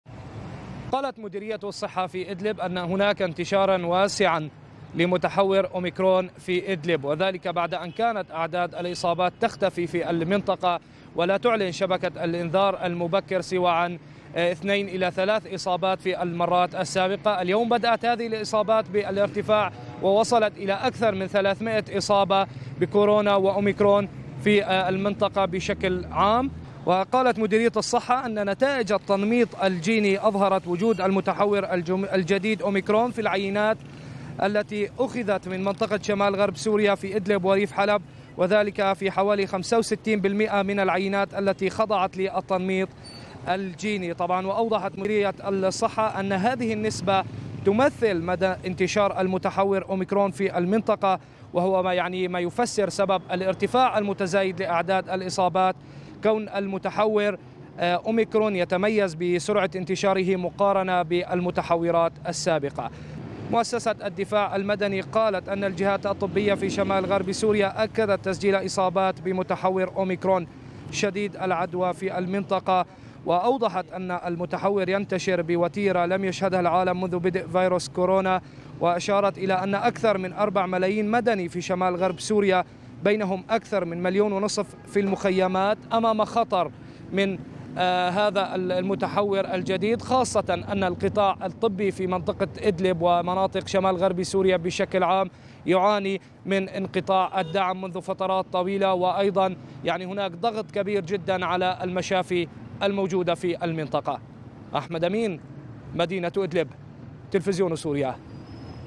صحة إدلب: متحور "أوميكرون" موجود بنسبة 65 في المئة من العينات التي خضعت للتنميط الجيني
كشفَتْ مديريةُ صحةِ إدلب بأنَّ نتائجَ التنميطِ الجيني أظهرَتْ وجودَ المتحوّرِ "أوميكرون" في العيّناتِ التي أُخذَت من مِنْطقةِ شَمالِ غربي سوريا في كلٍّ من إدلب، وريفِ حلب.